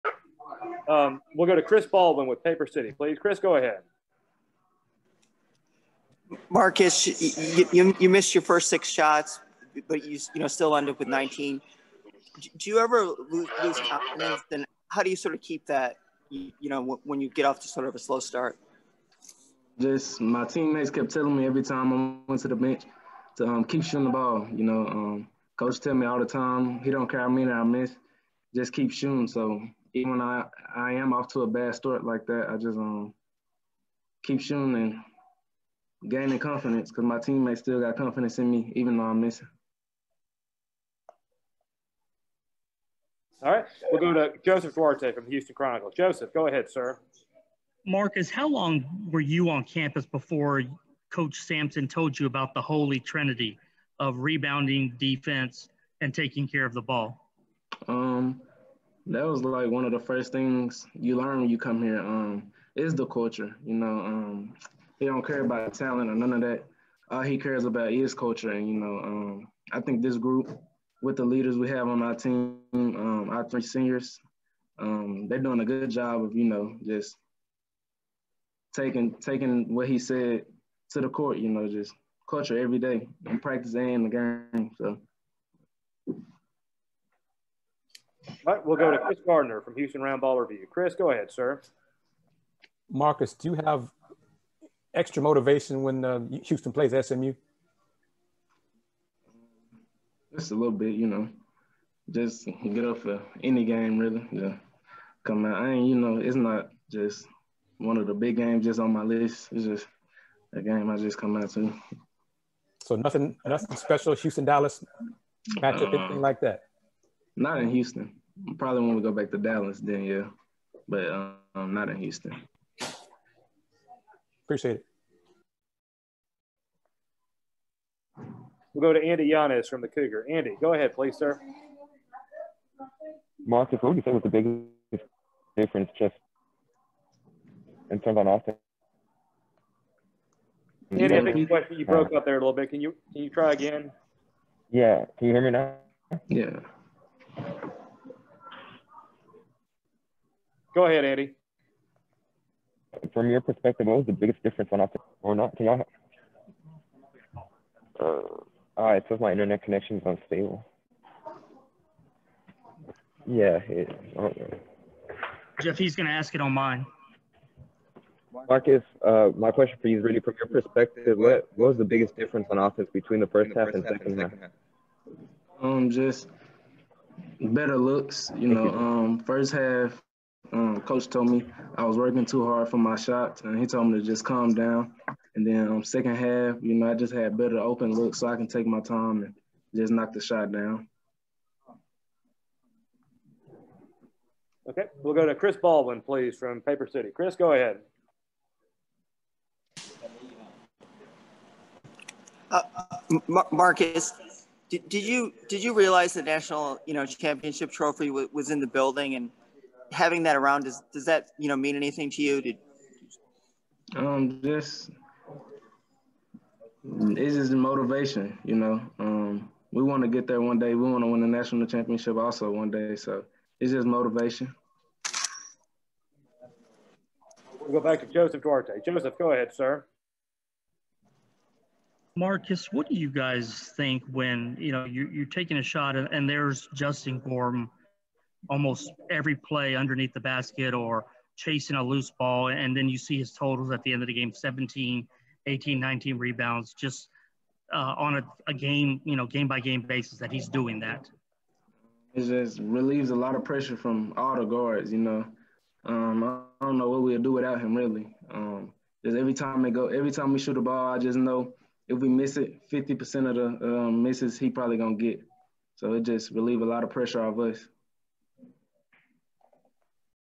Marcus Sasser: "That's one of the first things you learn when you come here is the culture"
Marcus scored 19 points in Houston's 70-48 win over SMU.

Marcus spoke with the media after the game about not losing confidence in himself if he's missing shots. He's teammates have confidence in him and encourage him to keep shooting the ball.

Marcus also discusses the Coach Sampson / Houston Cougars men's basketball "Holy Trinity": defense, rebounding, and taking care of the ball.

Marcus admits he has a little bit more motivation to face SMU - especially in Dallas.

Marcus says he got better open looks in the second half than he had in the first half.

Marcus says the national championship trophy being at Fertitta Center is motivation for the team.

Marcus says Justin Gorham's relentless rebounding relieves pressure on the guards.